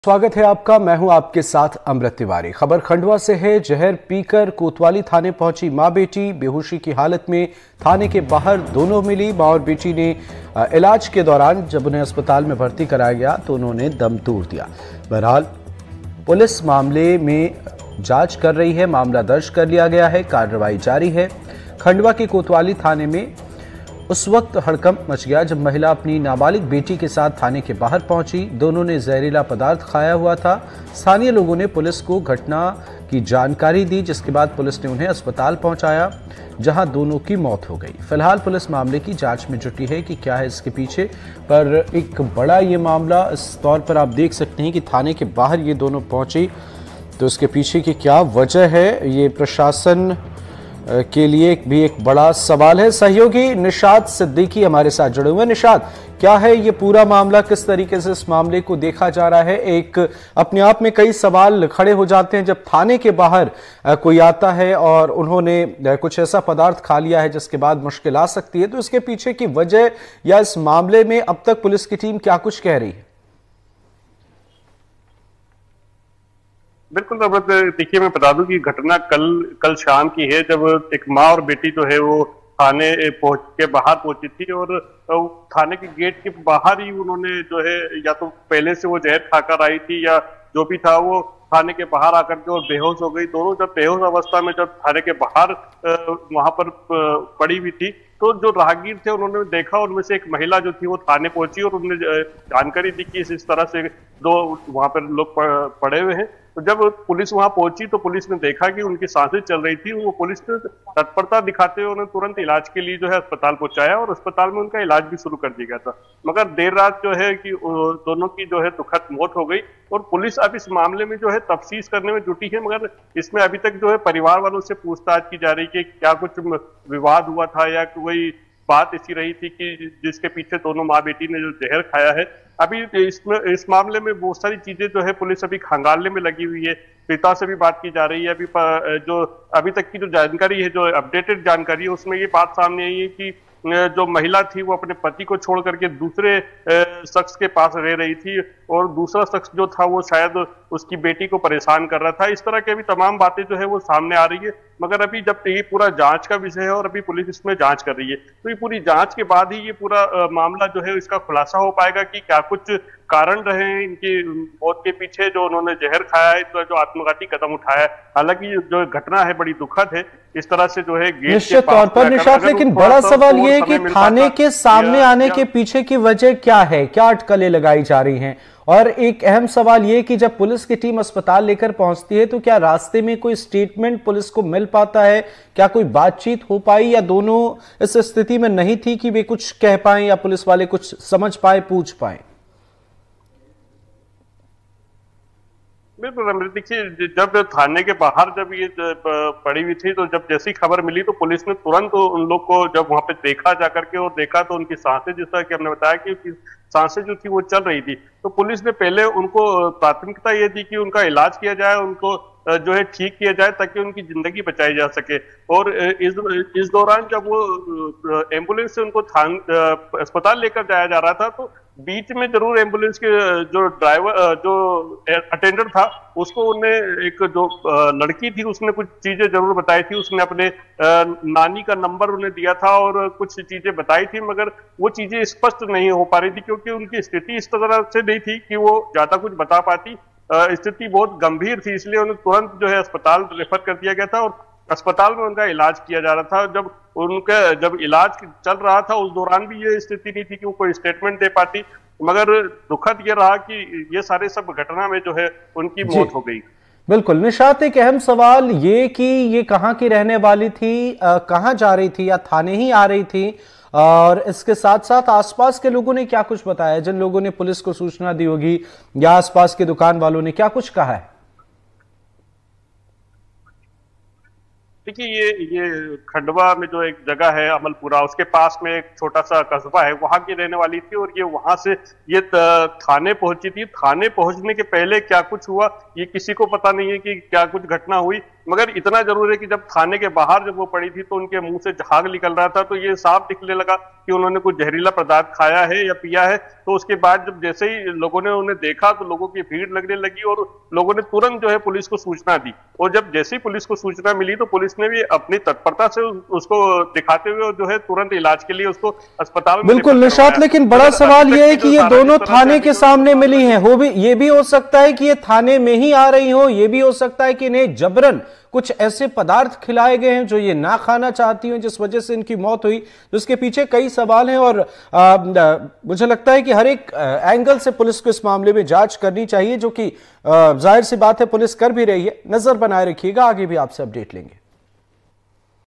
Khandwa में जहर पीकर थाने पहुंची मां -बेटी, Hospital में तोड़ा दम । Madhya Pradesh
खंडवा में (Khandwa) जहर पीकर कोतवाली थाने पहुँची माँ बेटी.  बेहोशी की हालत में थाने के बाहर मिली.  दोनों को बेहोशी की हालत में  इलाज के दौरान अस्पताल में भर्ती कराया गया तो उन्होंने दम तोड़ दिया.